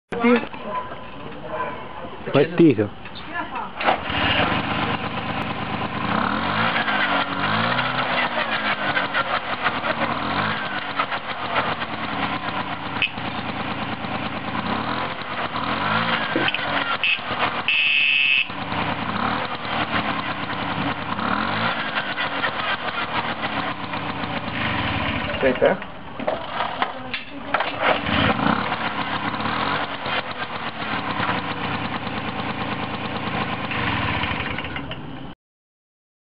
Ciao acceso del 1969 6-11-2010LowQ.flv
Il mio ciao piaggio del 1969 con variatore ancora perfettamente funzionante.
Dopo oltre 41 anni si accende al primo colpo, cammina benissimo e consuma a occhio e croce 1L x 30-40km! leggero e totalmente originale tranne pedali e marmitta (che ovviamente l'originale è deceduta nel tempo). Ancora libretto e le chiavi sono da cercare (se ci sono sempre).
E' stato fermo 10 anni, ma tolto benzina e messa nuova, pulito la candela e carburatore ecco che è partito senza colpo ferire!